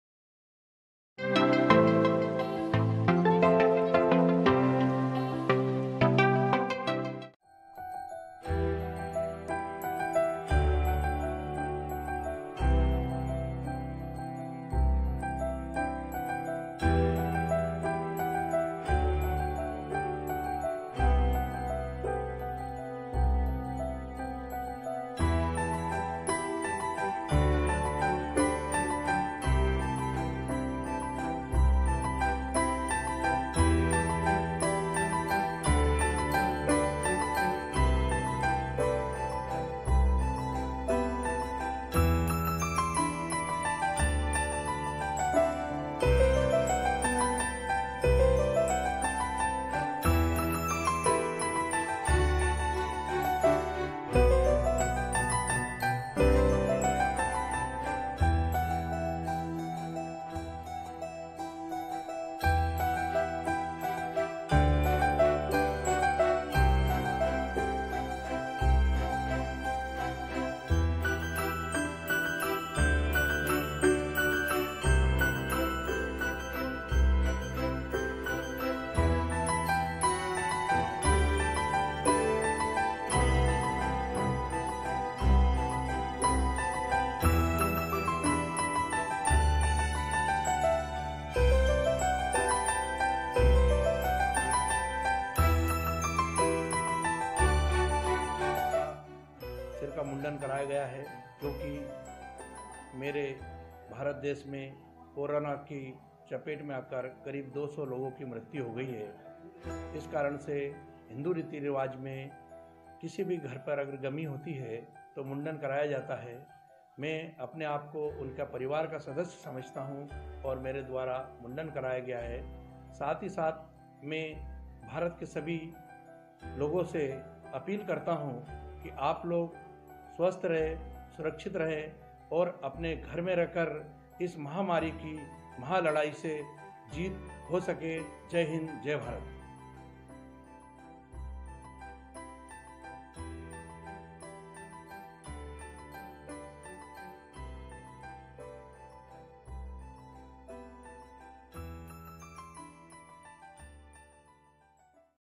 Khandwa: Corona से मरने वालों को पुलिस ने सिर मुंडवा कर दी श्रद्धांजलि
बोरगांव बुजुर्ग पुलिस चौकी प्रभारी उप निरिक्षक राधेश्याम मालवीय मुंडवाया सिर... सिर मुंडवाने का वीडियो सोशल नेटवर्किंग साइट्स पर किया वायरल...  भारत में कोरोना से मरने वालों को दी श्रद्धांजलि...